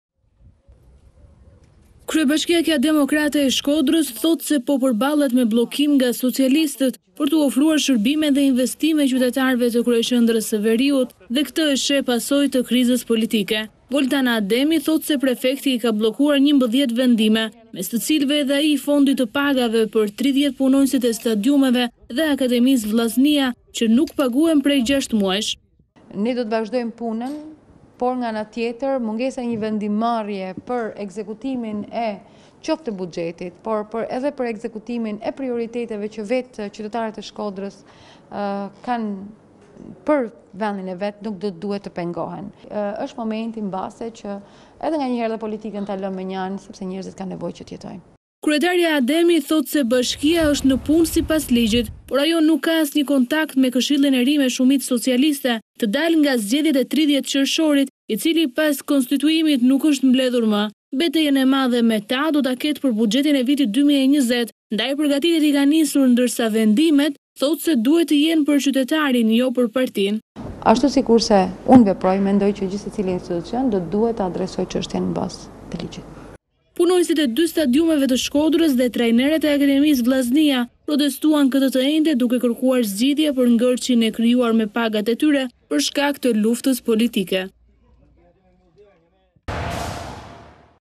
RTV Ora - Punonjësit e KF Vllaznia pa rrogë, Ademi: Fondet janë miratuar, pritet vendimarrja
Kryetarja në detyrë e Bashkisë Shkodër, Voltana Ademi ka reaguar pas protestës së punonjësve të klubit të Vllaznisë, të cilët prej 6 muajsh nuk paguhen.

Ademi tha se shqetësimi është ngritur disa herë nga ana e Bashkisë. Ajo shtoi se fondi është në dispozicion por është bllokuar nga prefekti i Shkodrës dhe pa një vendimmarrje nuk mund të përdoret për pagimin e punonjësve.

Voltana Ademi: "Shqetësimi është ngritur shumë herë. Situata ku ndodhet bashkia është pak komplekse dhe lidhet me krizën politike. Këshilli Bashkiak ka 3 muaj që nuk merr asnjë vendim. Në bashkinë Shkodër nuk ka një vendimarrje që lidhet me vijueshmërinë e punës për tu ekzekutuar nga ekzekutivi, administrata e bashkisë Shkodër dhe për pasojë ka edhe pengesa që vijnë në shërbimin që ne realizojmë.